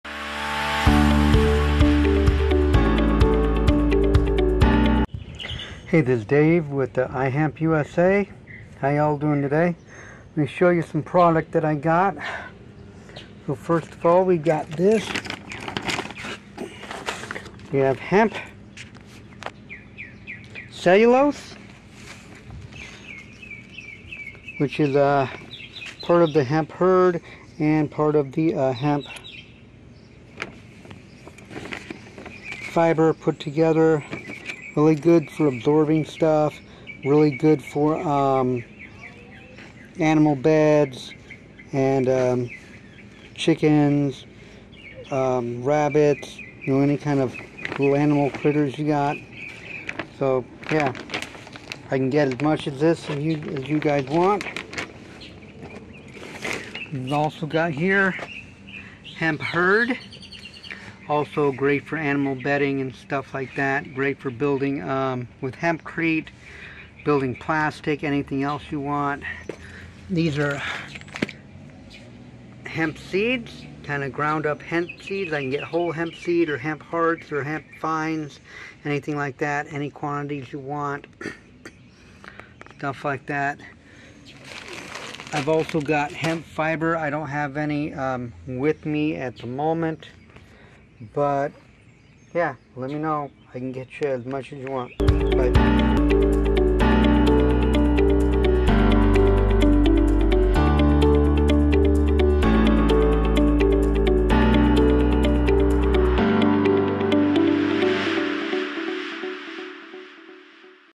Stuff I sell
This is just some of the new products that I sell.
Hemp Hurd, Fiber, Cellulose,  In Bulk Seeds, and oil.